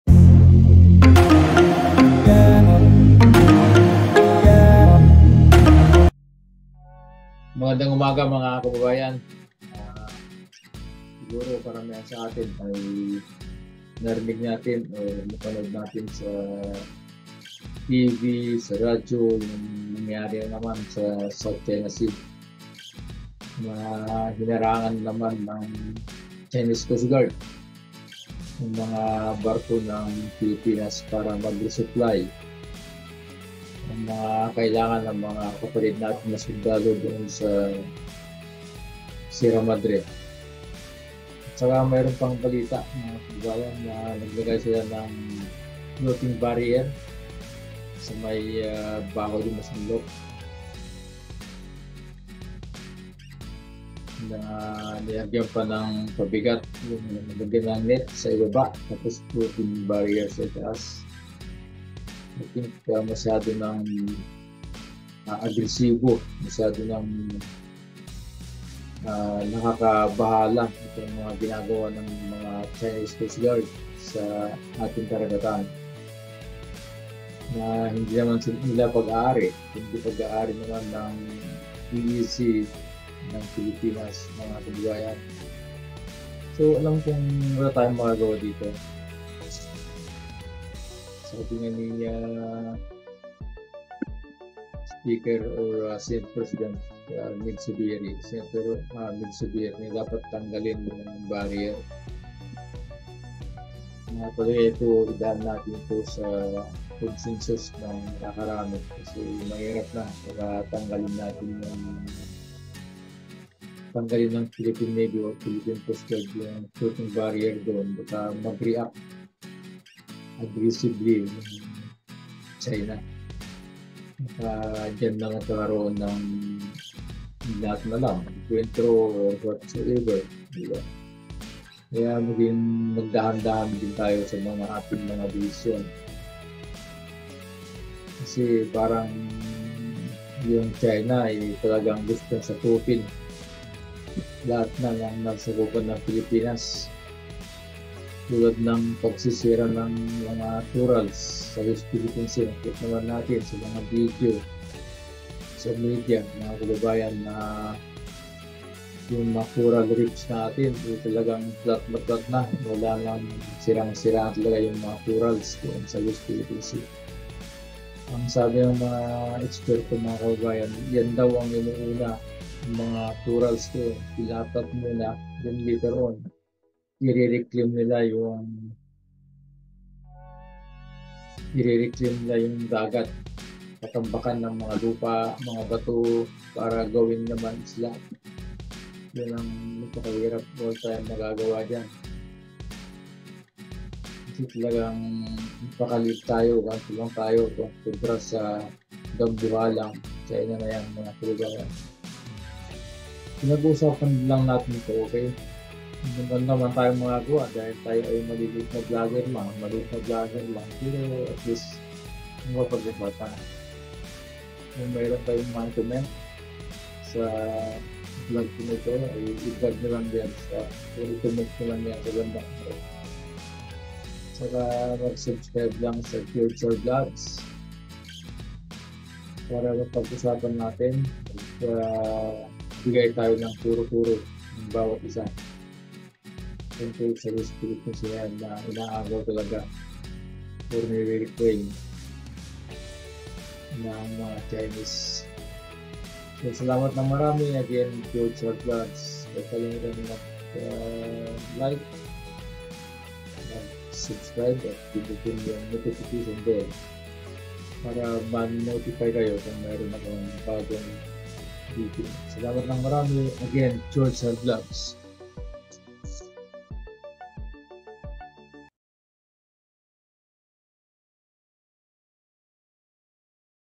West Philippine Sea Talagang Gusto Ng Sakupin Ng China II Floating Barriers Nilatag Ng China
West Philippine Sea Talagang Gusto Ng Sakupin Ng China II Floating Barriers
Nilatag Ng China